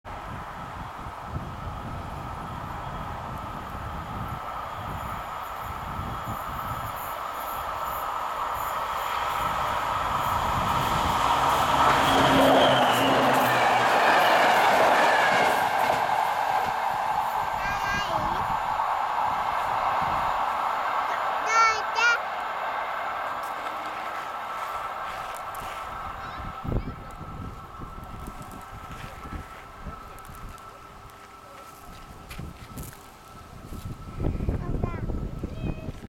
2011-08-21 [SNCB/NMBS] Class AM 80, L 2464
ベルギー国鉄AM 80形電車によるローカルトレイン2464。2011年8月21日、ルーヴェン駅－ウェーゼマール駅間（ベルギー）にて撮影。
L (Local train) 2464 using the Class AM 80 electric multiple unit (EMU) of SNCB/NMBS (Société Nationale des Chemins de fer Belges/Nationale Maatschappij der Belgische Spoorwegen, Belgian National Railways). Recorded between Leuven Station and Wezemaal Station in Belgium on August 21, 2011.